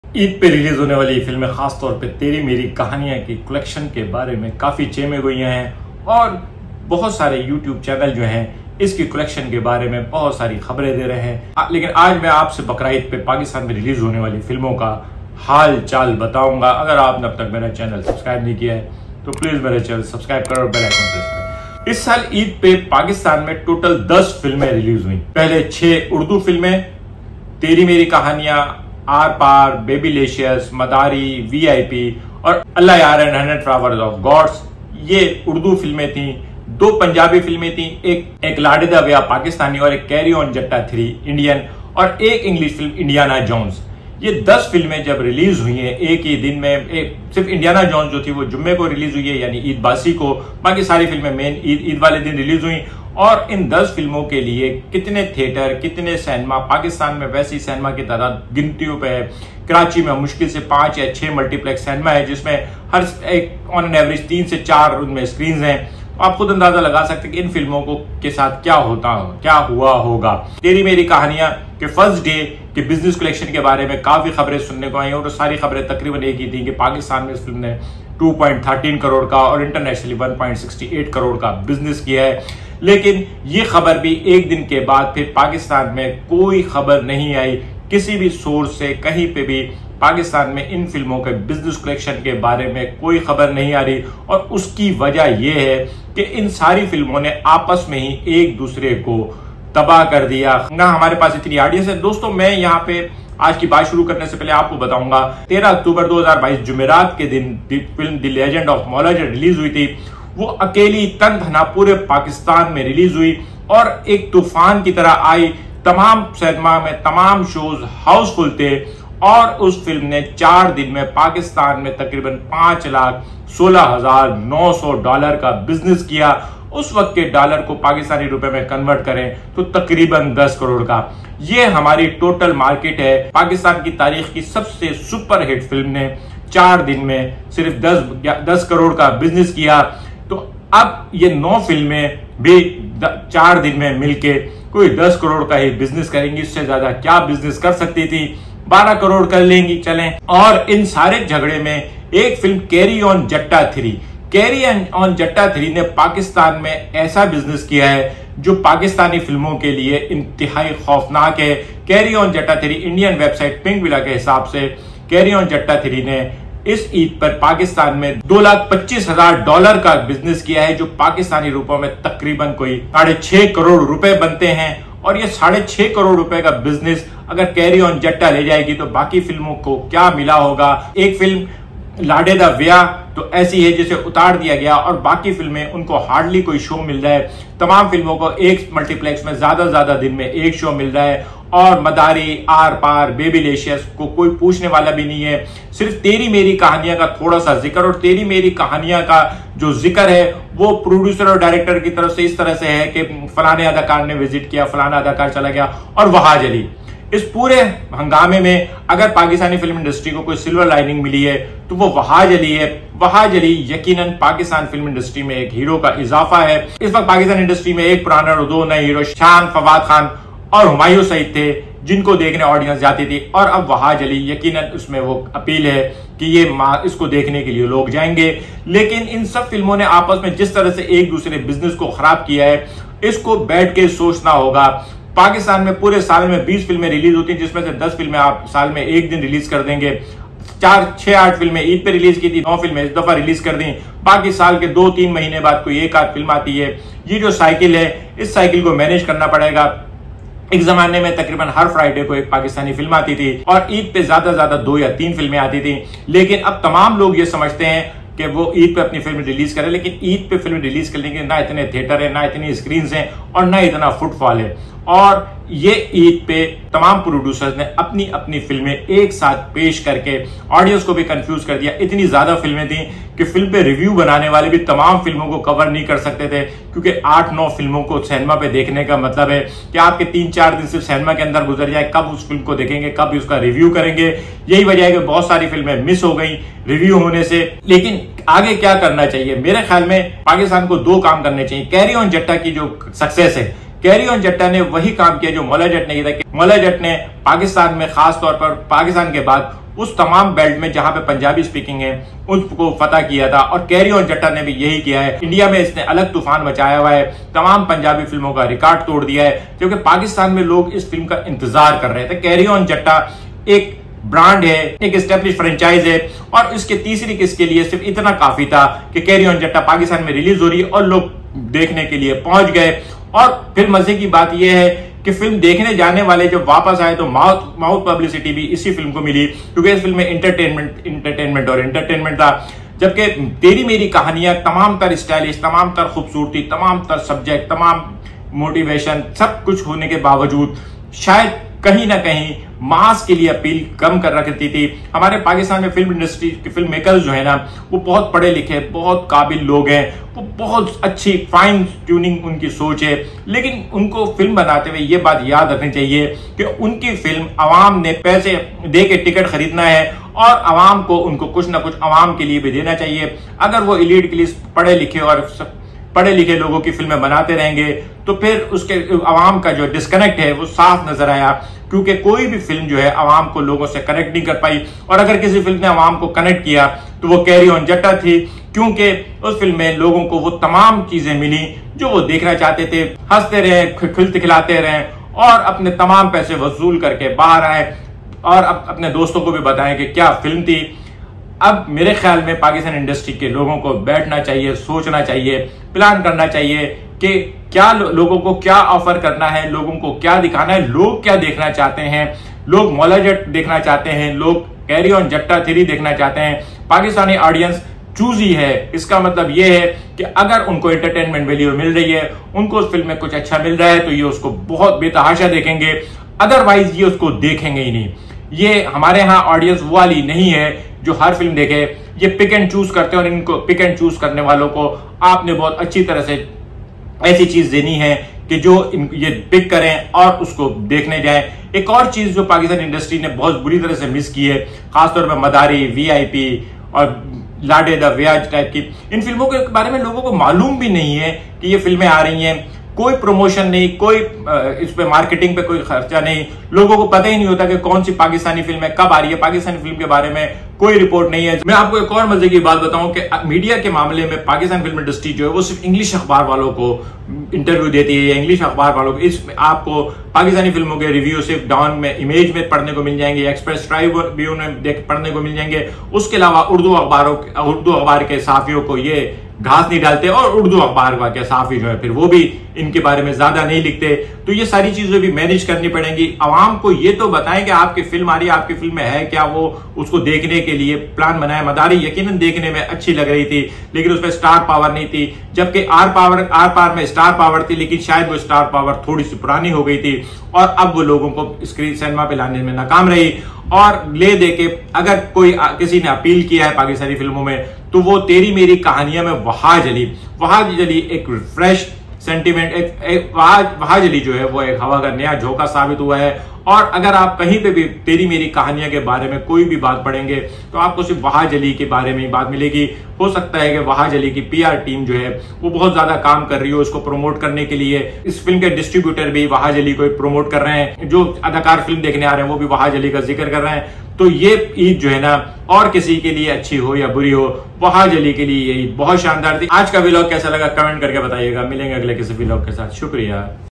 Teri Meri Kahaniyaan & Other Eid Release Collection [Update]
Teri Meri Kahaniyaan Collection & Other Eid Release Box Office Collection:
On this Eid Total 10 films released on Cinema, Teri Meri Kahaniyaan, Aar Paar, VIP, Babylacious, Madari, Allahyar & 100 Flowers of God, Carry on Jatta 3 (Indian Punjabi), Laade da Viah (Pakistani Punjabi) and Hollywood Adventurer Thriller Indiana Jones and the Dial of Destiny.

This number is very large as compare to size of cinema goers in Pakistan and hence none can do remarkable business except Carry On Jatta 3.

An update and analysis